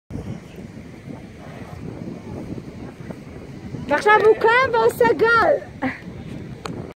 חוף תל אביב - אטריקציה לאירועים - כדור בועה אנושי 0532304420
משחק אדיר כדורגל בתוך בועה
על חוף הים
KSUMIM.CO.IL

קישור לרכישה